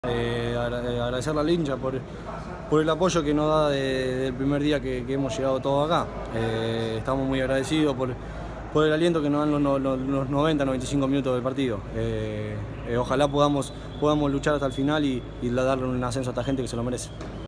Mensaje del goleador Bohemio Luciano Pons
Tras la victoria 2-1 sobre la hora ante Dep. Armenio, el goleador Bohemio, Luciano Pons, dejó su mensaje para los hinchas Bohemios.